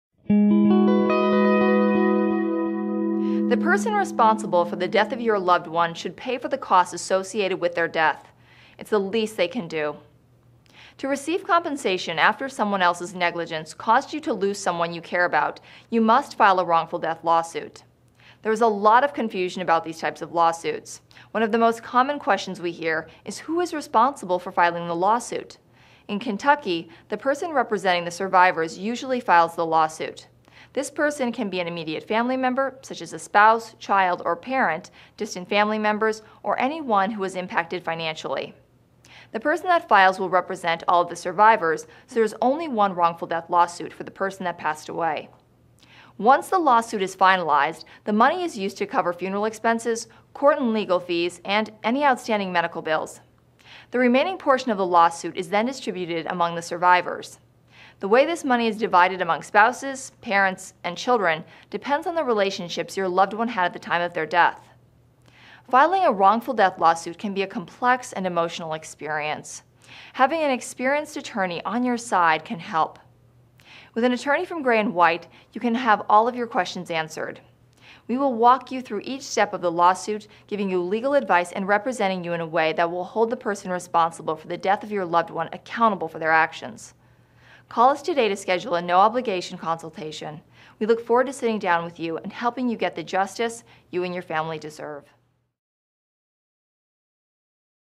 Who Can File a Louisville Wrongful Death Lawsuit?
An unexpected death can leave a family in mourning. When someone you love is suddenly taken from you due to another's negligence, you need to find out as much as you can about pursuing a wrongful death suit. One of the most common questions our Kentucky wrongful death lawyers receive is about filing the suit. Unfortunately, there is confusion surrounding these types of lawsuits, and survivors aren't sure which one of them can bring a suit.  In Kentucky, there can only be one wrongful death lawsuit, and it is typically filed by the person representing the survivors. The survivors can include a spouse, child, parent, immediate family member, or anyone impacted financially by the death. The money collected will pay for any outstanding medical bills, funeral costs, and legal fees, and then it will be divided accordingly between the survivors. Filing a Kentucky wrongful death lawsuit can be an emotional time but necessary to secure the compensation and justice a family deserves.  For help holding the person responsible for your loved one's death, contact an experienced Louisville wrongful death attorney at Gray and White Law. Our lawyers will help you hold the wrongdoer responsible.